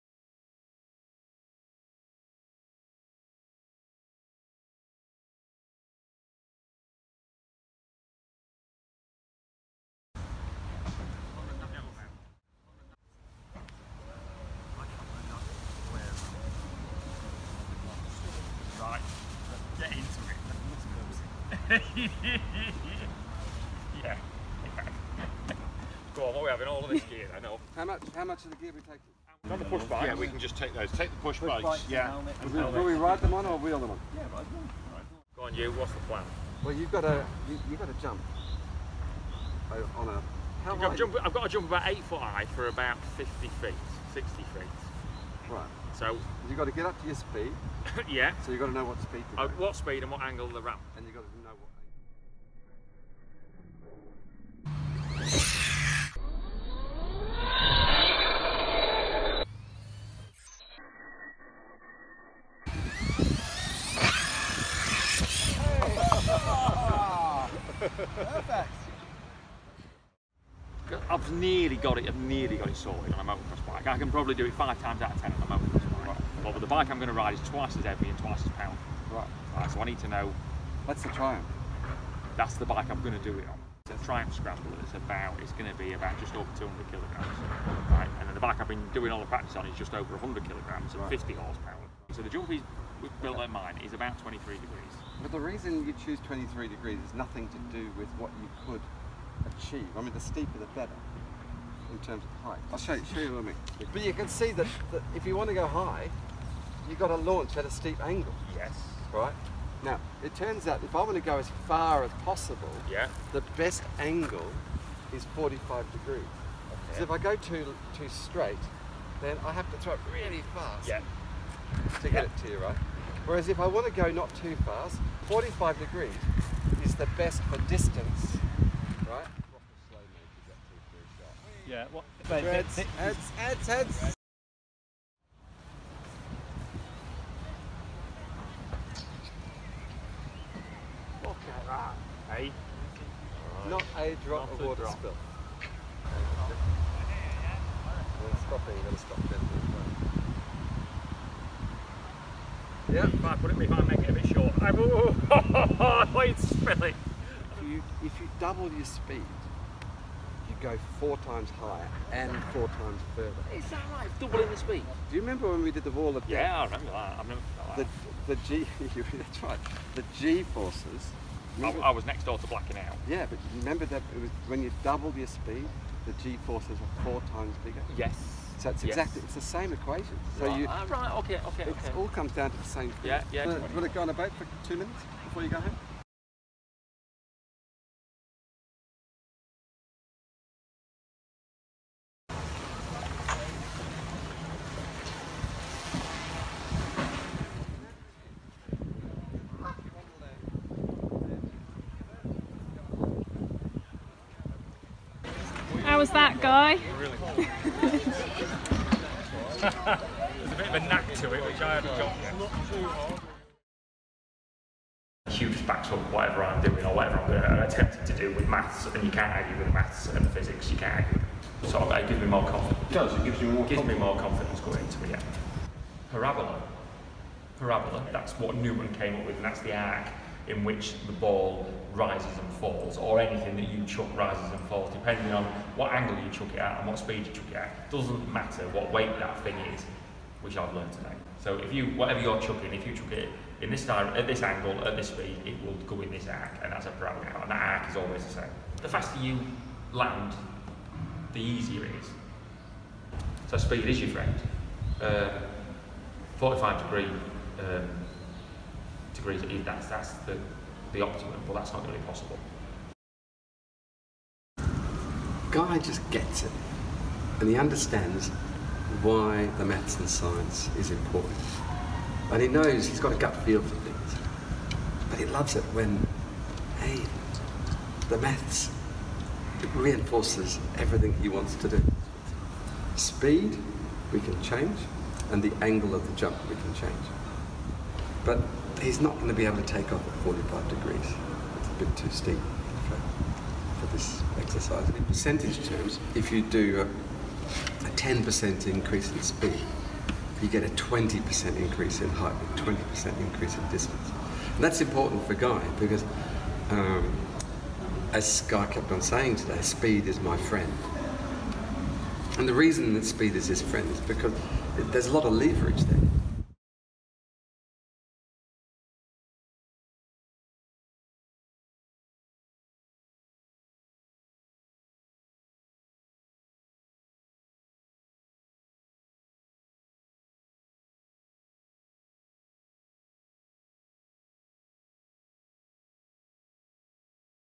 When Guy Martin came to Cambridge
The Great Escape film is considered to show one of the most iconic motorcycle stunts in movie history. Now, nearly 60 years later, Cambridge engineer Dr Hugh Hunt helps motorcycle racer Guy Martin prepare to recreate the jump – with a little help from physics. Guy visited Trinity College Cambridge to meet with mechanical dynamics expert Dr Hunt, as part of a new TV programme, looking at the real events that inspired the 1963 movie.